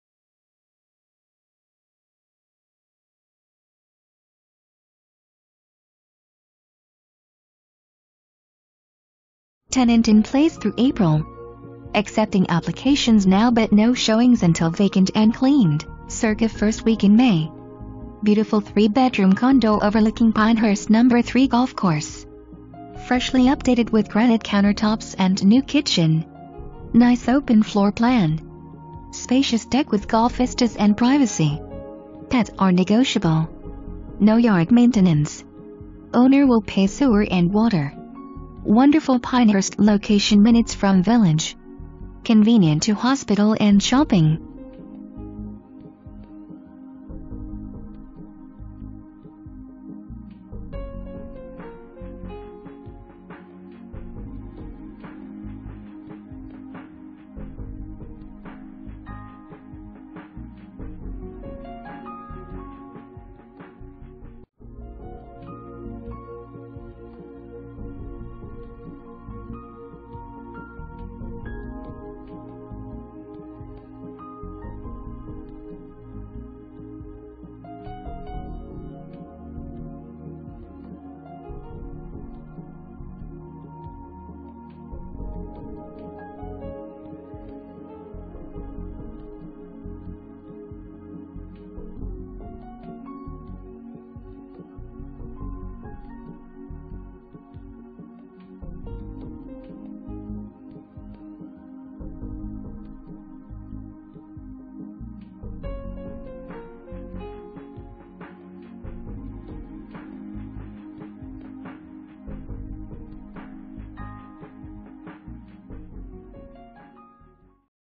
Homes for Sale - 44 Quail Hill, Pinehurst, NC
Tenant in place through April. Accepting applications now but no showings until vacant and cleaned, circa first week in May.   Beautiful 3 bedroom condo overlooking Pinehurst No. 3 golf course. Freshly updated with granite countertops and new kitchen.  Nice open floor plan. Spacious deck with golf vistas and privacy.  Pets are negotiable.  No yard maintenance. Owner will pay sewer and water.  Wonderful Pinehurst location minutes from Village. Convenient to hospital and shopping.

Presented By:
Bob Barmore, Coldwell Banker Advantage